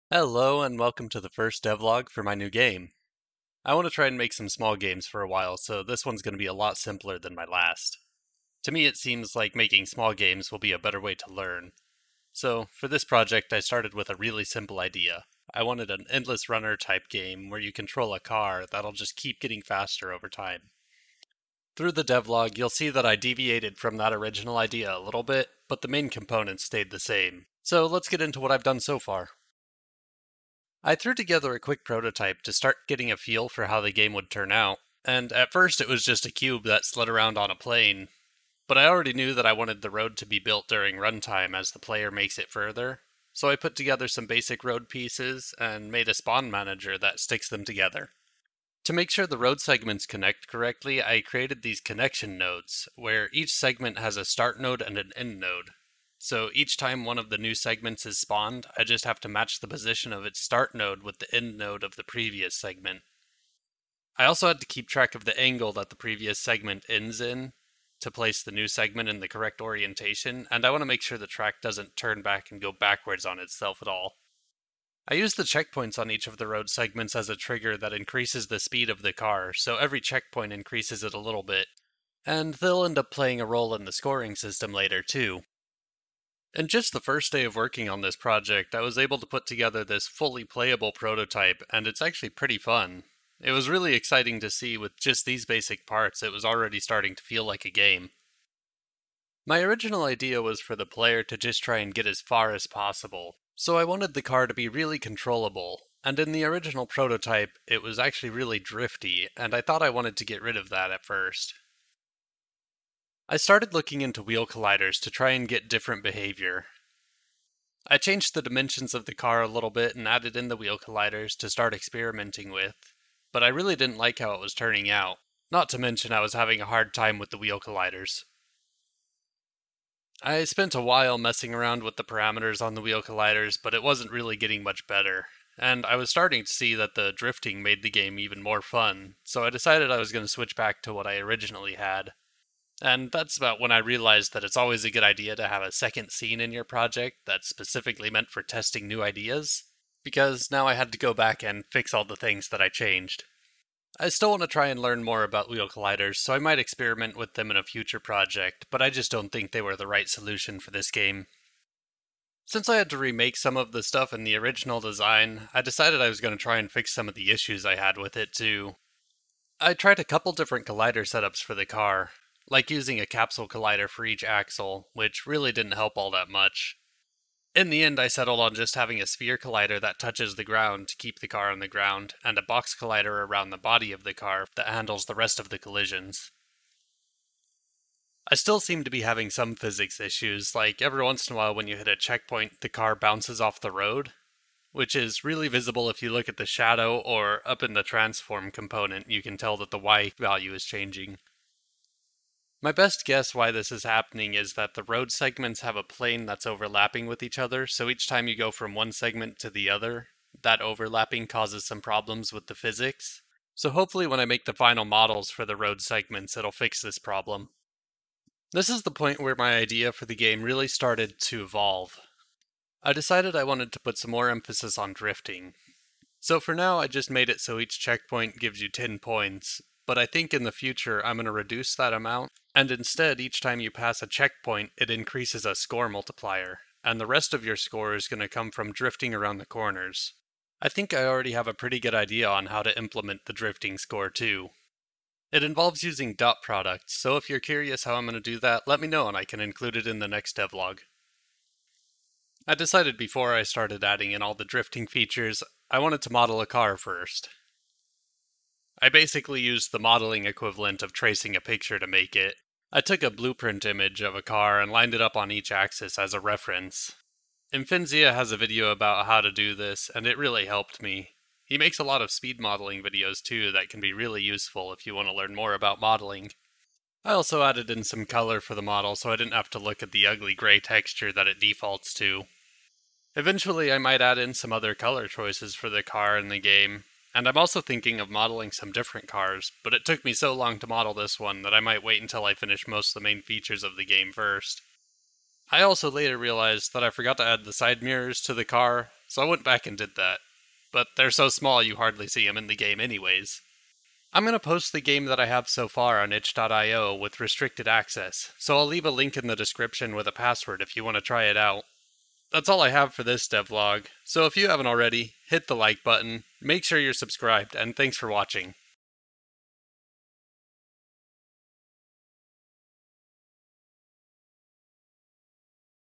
Endless Drift Devlog 1 | Building the prototype
This is the first devlog for my new game, Endless Drift. I go through building an early prototype for the game and discuss what my plans are for the future of the project.

Password: soda